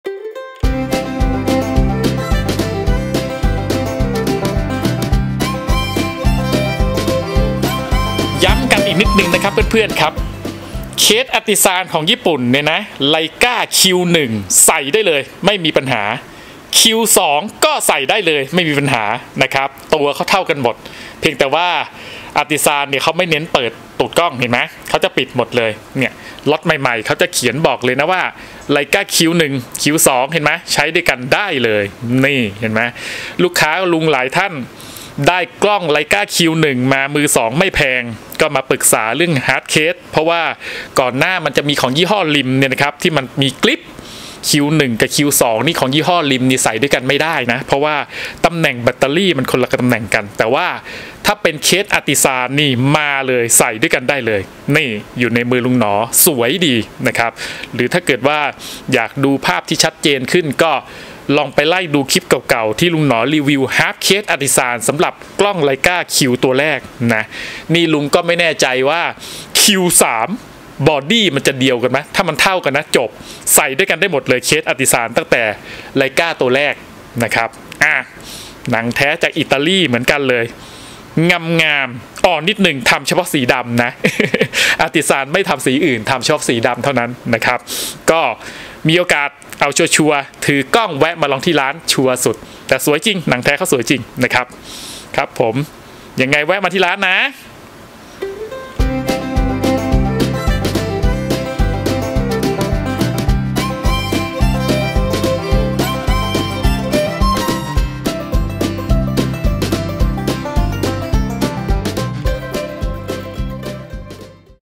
รีวิวเคสกล้อง Leica Q2
รีวิวเคสกล้อง Leica Q2 แวะมาที่ร้านลุงหนอนะครับ
Nhor Shop